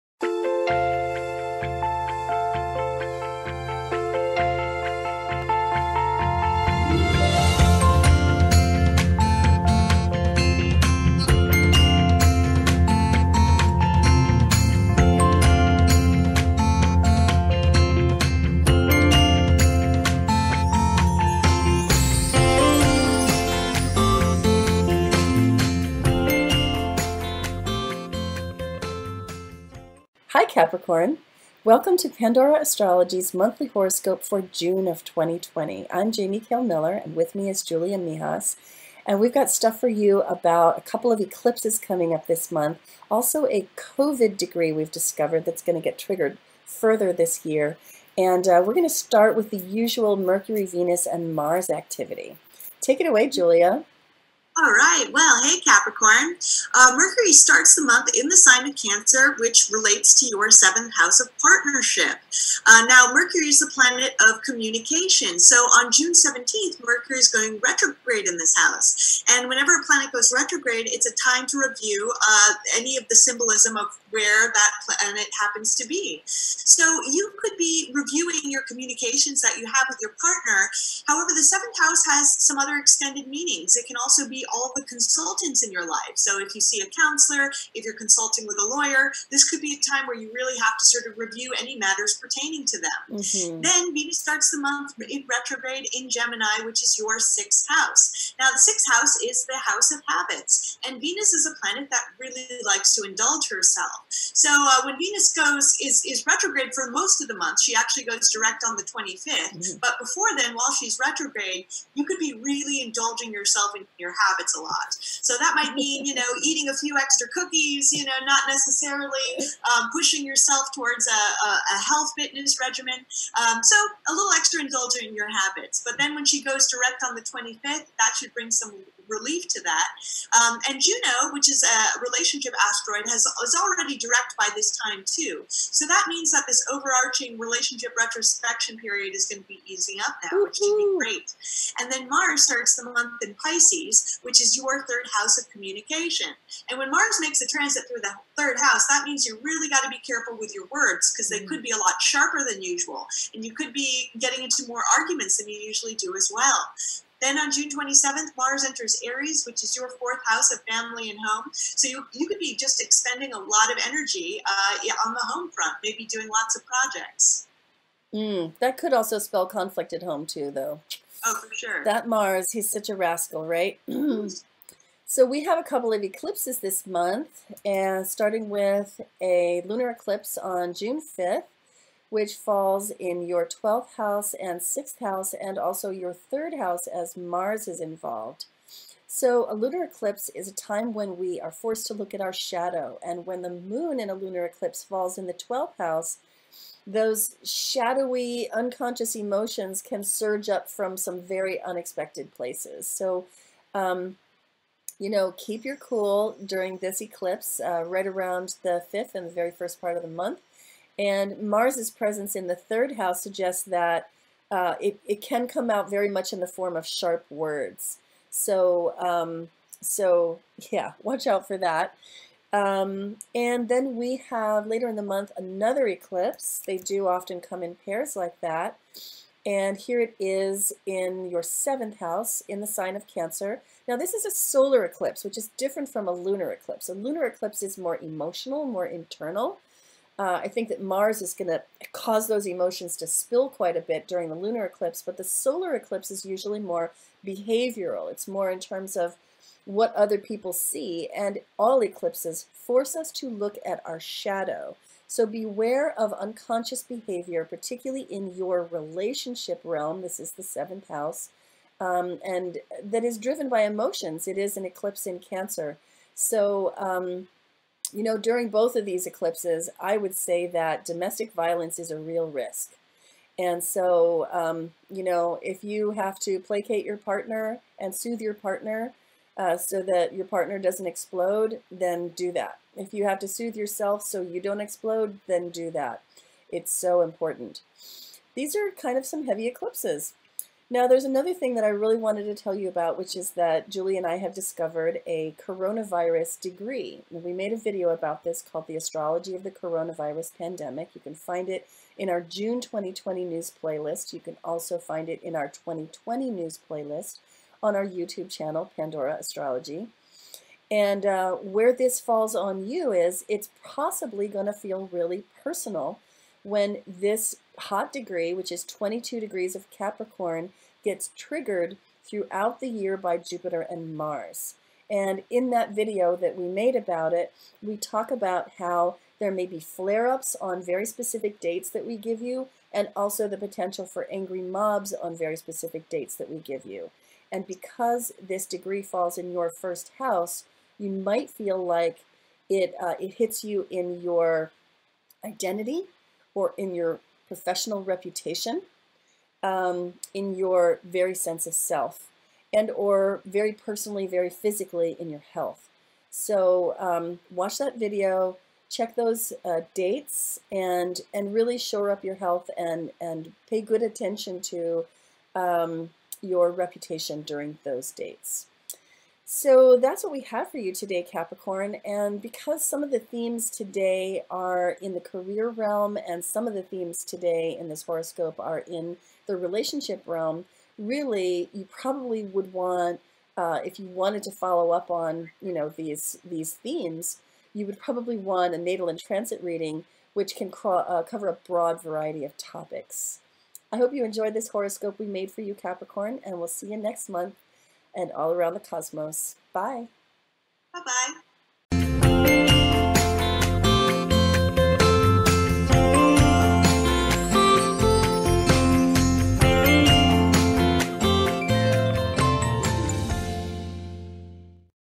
Capricorn Horoscope June 2020 | Your Integrity Is Everything | Pandora Astrology
Hi Capricorn!
June begins with a Lunar Eclipse in your 12th House of the Unconscious. This is likely to bring up emotions you didn’t know you had and that can lead to arguments under stress—so cool your jets. In the middle of the month, Mercury goes retrograde in your 7th House, causing you to reevaluate communications with partners, agents and legal counsel. What did they really mean by that? Additionally, a Solar Eclipse happens there at month-end, so don’t let emotions trigger unconscious reactivity in your relationships. The best news is that Venus ends her part of the “relationship retrospection” period going on this spring, so your relationships should start to untangle, particularly at work. During this spring you may be discovering that your health habits are overly pleasure-driven, so put into place some changes to your routines and cut out the indulgences. And most importantly, we have discovered a Pandemic Hot Degree and it’s in YOUR sign. This is the year in which you must take responsibility for things that are beyond your control and handle yourself with grace, not victimhood or bitterness. Your integrity matters more than anything. Draw on the Serenity Prayer as needed.

If Capricorn is your Sun sign, be sure to go watch the video for your Rising sign, which will be more on point.

This month your horoscope’s themes are a mixed bag. You might choose a Mission Orders career reading or a Lifelong Love reading, depending on which matters most to you right now. You’ll find them here:

To find out what I mean by a “Pandemic Hot Degree,” visit our 2020 news playlist and watch “The Astrology of the Coronavirus Pandemic”

You’re shinier than you think, Capricorn!
-Jamie and Julia, the Pandora Astrology Team